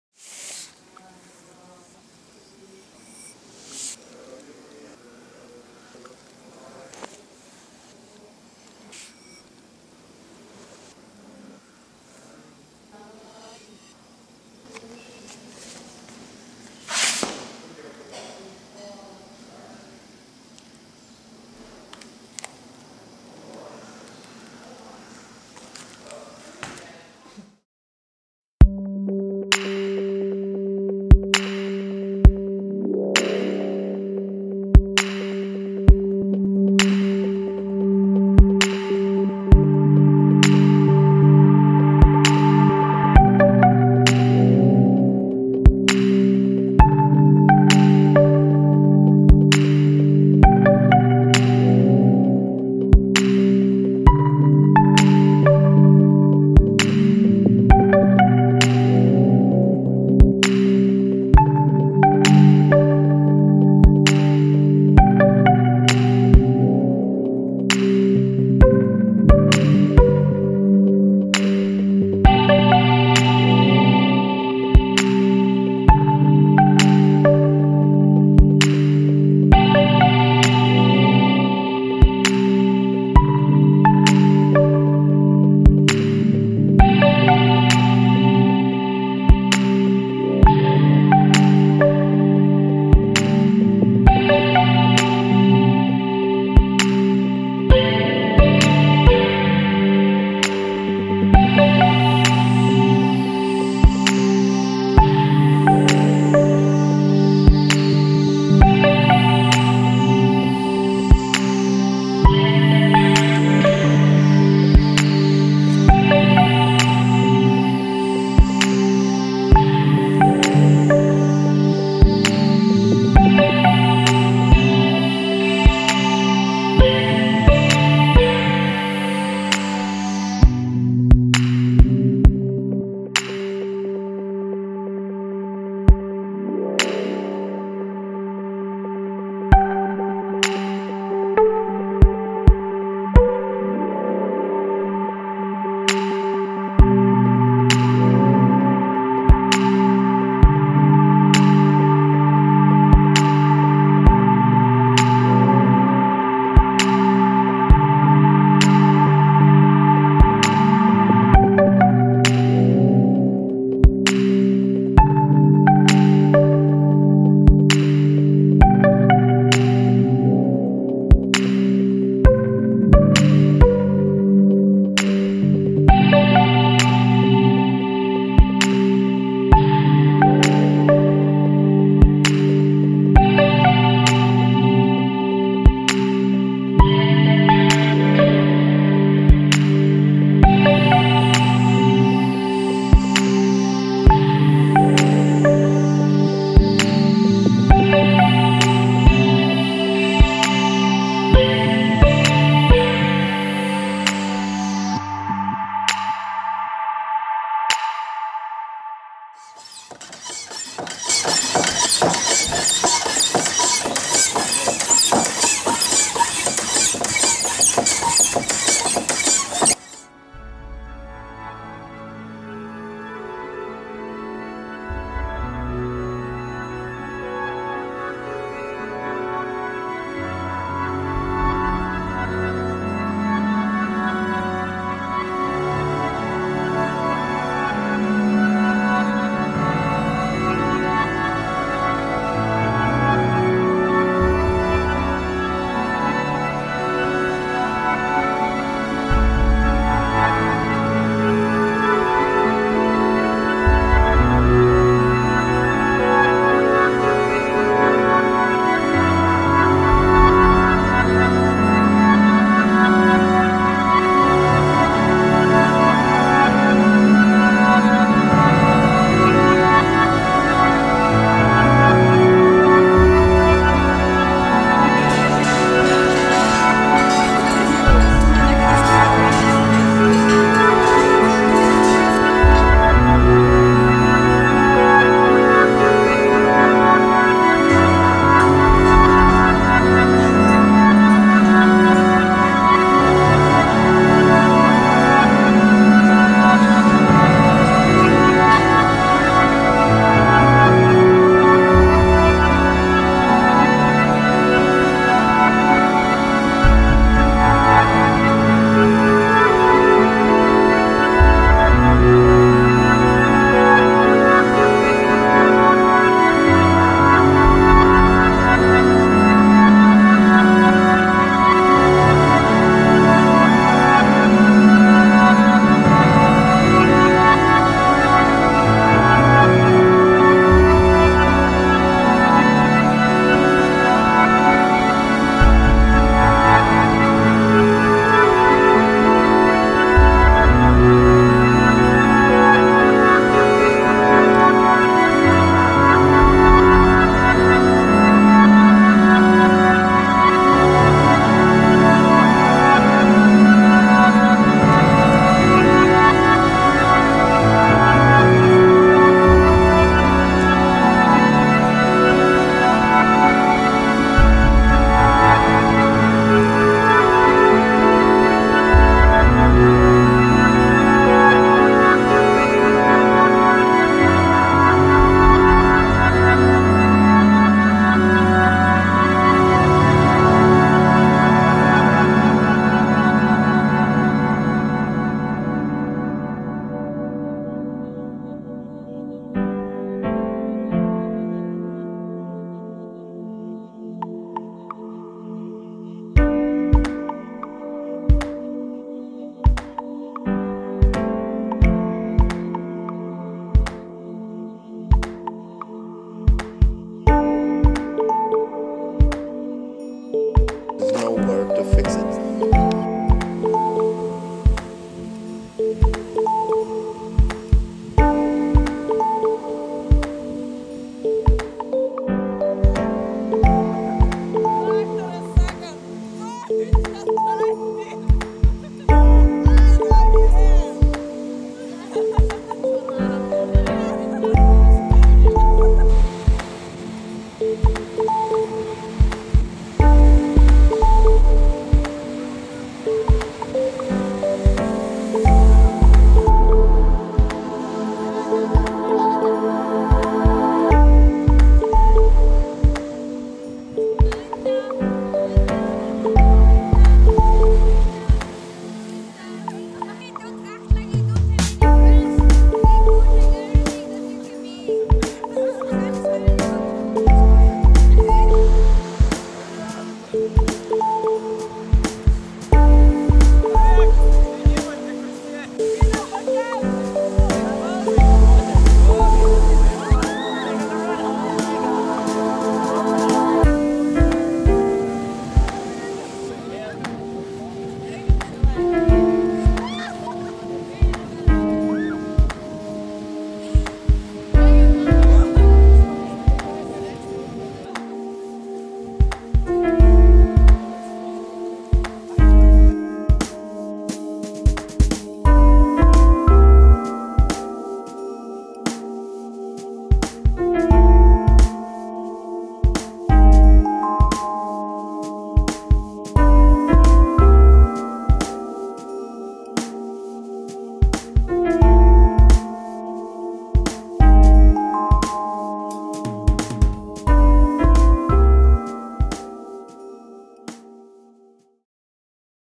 Dérive Summer Media Studio 2016 by Jeannette Ginslov
A short dérive of the Summer Media Studio at Nida Art Colony 2016 in Nida Lithuania. How to make a building dance. A film by Jeannette Ginslov.

This was a two week screendance residency for artists and students where they explored the notion of dance on screen, interactivity in 360 degree films and augmented reality.